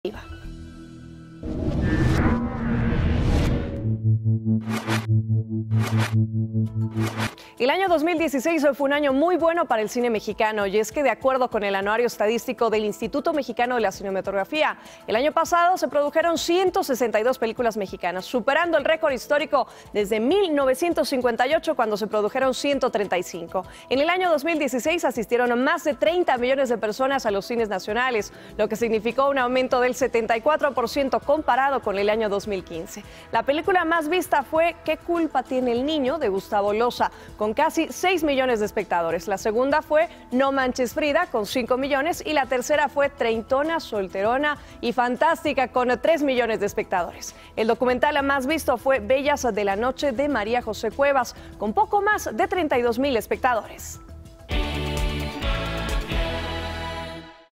Cine mexicano rompe récord
En 2016 México rompió récord de películas producidas. Igualmente la asistencia a funciones de películas mexicanas fue bastante alta.